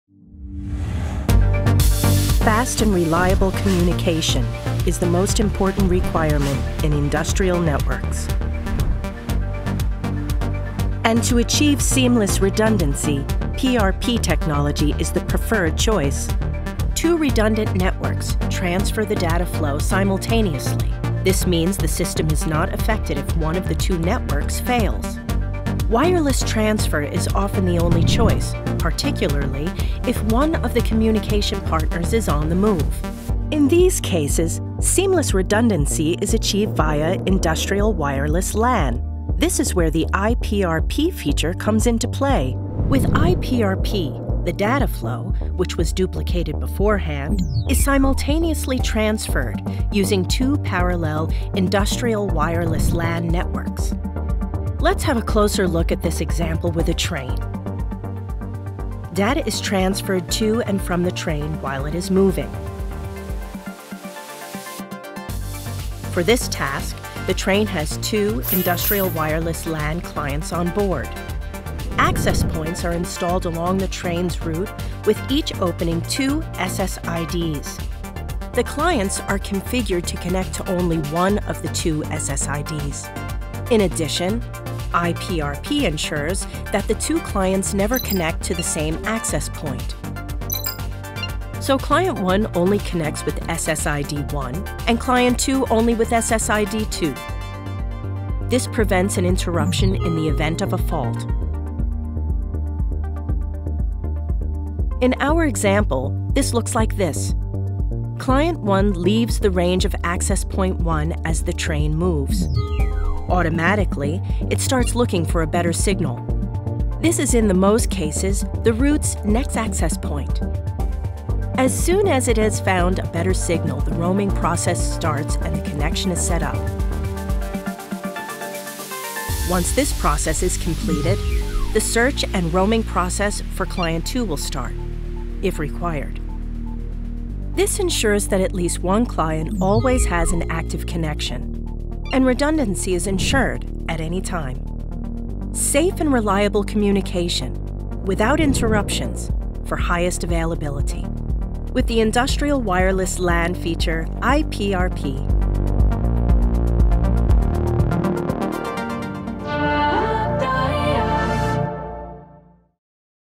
Seamless redundancy via IWLAN with iPRP
The IWLAN iFeature iPRP is the right solution for mobile communication participants and the highest demands on network accessibility. The video shows how iPRP enables an interruption-free communication.

In the application example shown, the following hardware is used:
Access Points:  SCALANCE W786-2 RJ45
Clients: SCALANCE W788-2 M12 EEC